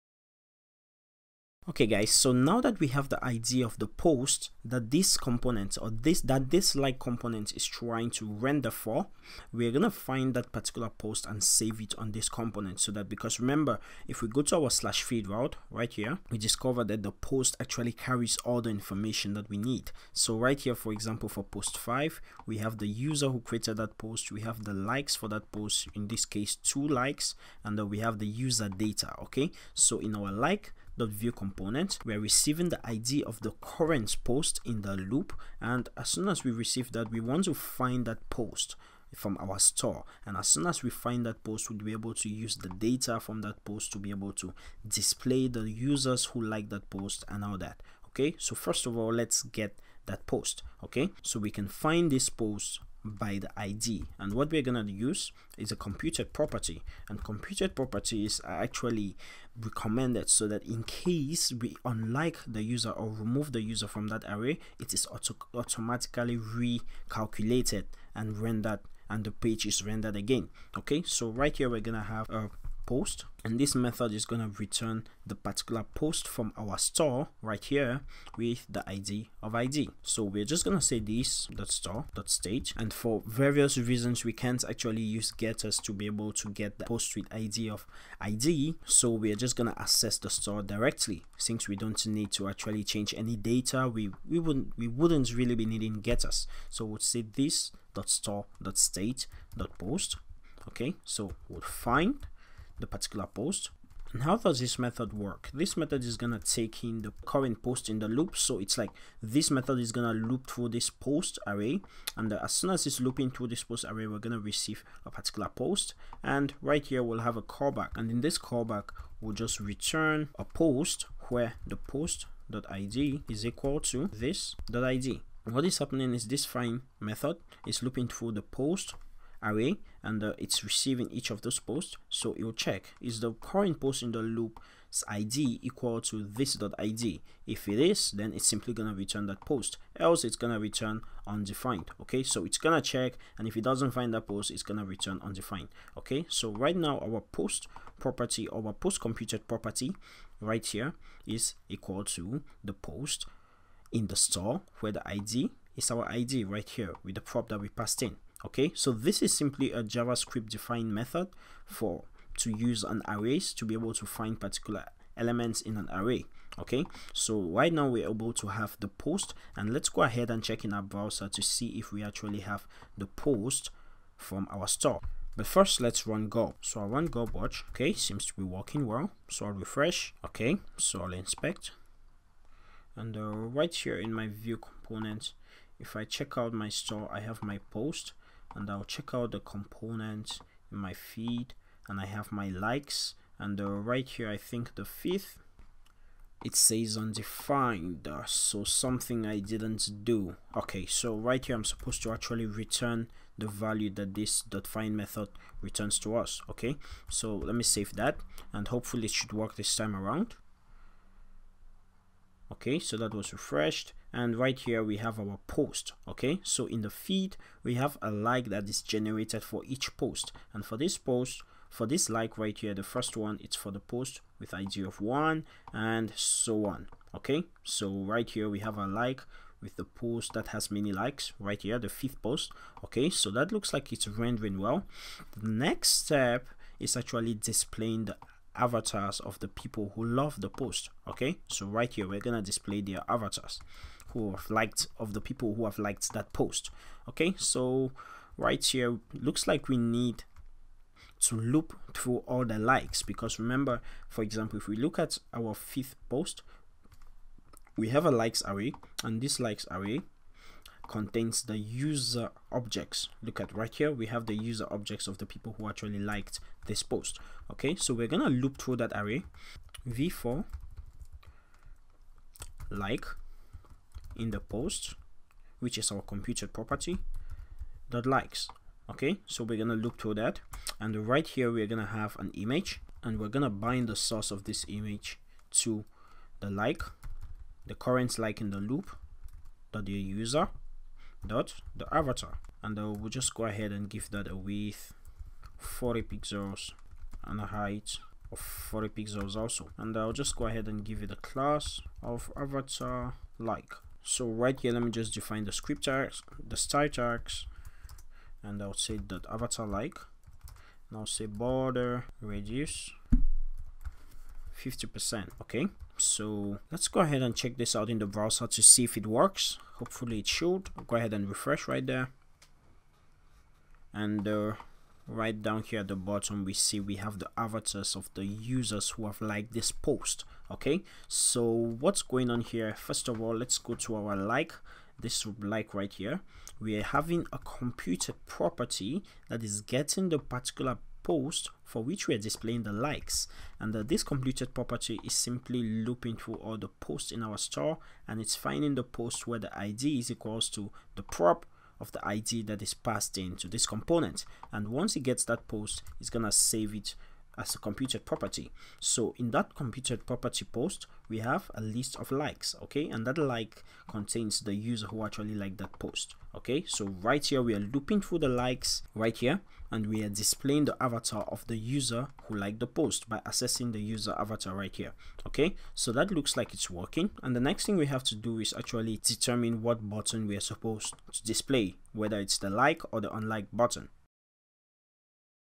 Social network in Laravel 5.3 and Vuejs 2.0 #46 Finding post from vuex store
Let's build a complete application with the Laravel 5.3 and Vuejs 2.0. In this video, we fetched post data into our Like.vue component